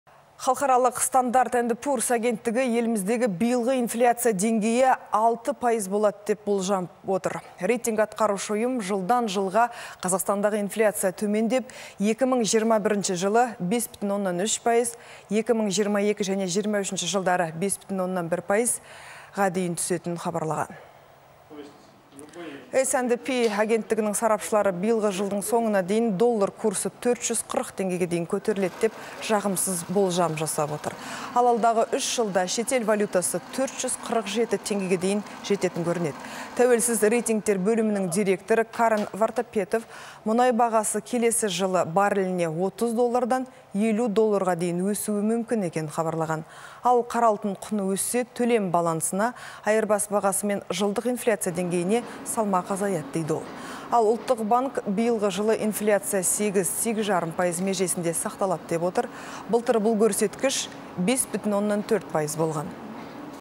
Қазақстандағы биылғы инфляция деңгейі қандай болады?
Халықаралық S&P агенттігі еліміздегі биылғы инфляция деңгейі 6 пайыз болады деп болжап отыр. Рейтинг анықтаушы ұйым жылдан-жылға Қазақстандағы инфляция төмендеп, 2021 жылы 5,3%, 2022 және 2023 жылдары - 5,1% дейін түсетінін хабарлаған.